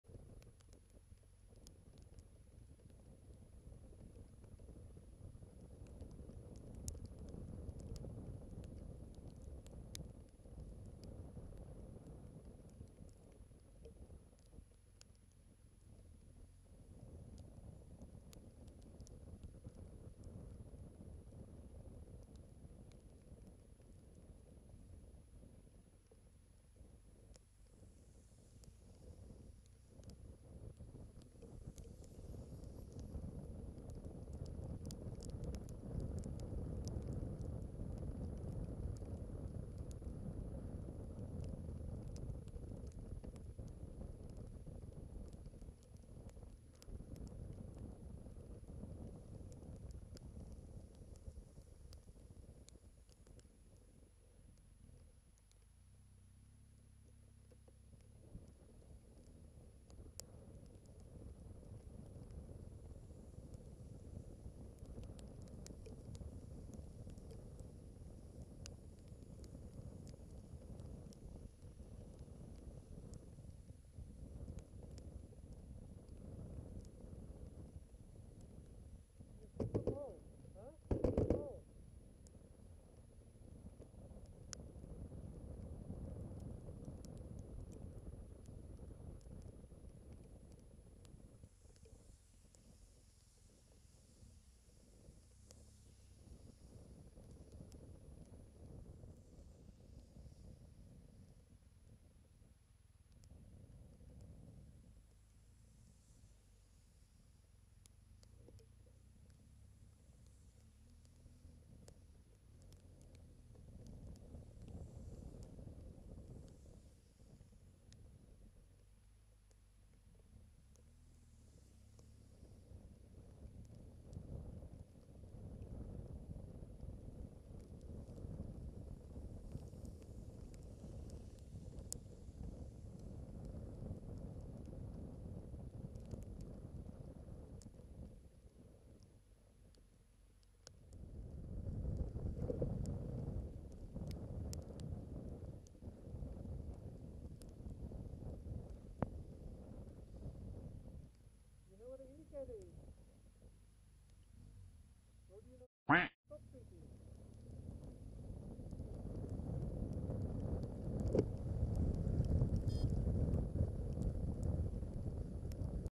Overtaken by 2 cars while lane splitting, in the rain and my way home
On my way home today, and the 21st time I got wet because of rain this summer. While slowly lane-splitting past cars I got overtaken by 2 cars. Registration numbers FT94FK-GP and KELE24-GP, and then a 3rd car just moves from lane to lane not using indicators, and ignoring STOP signs.